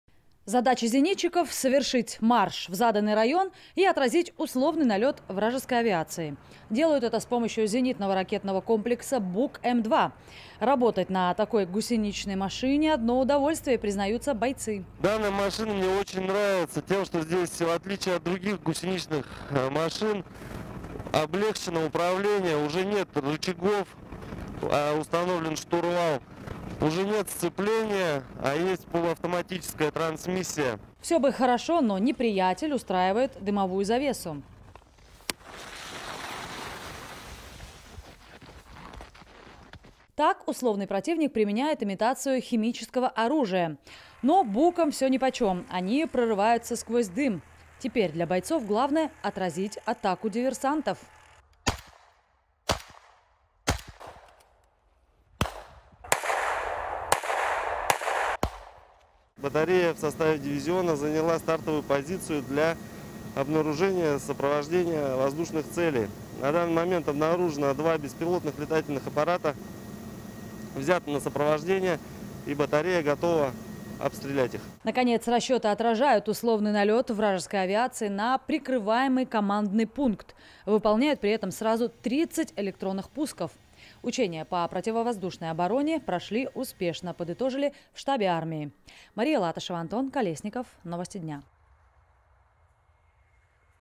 Отразили атаку
В бой идут "БУКи"! В Бурятии завершились учения зенитчиков на современных зенитно-ракетных комплексах. Как машины показали себя в условном бою, расскажем в следующем сюжете.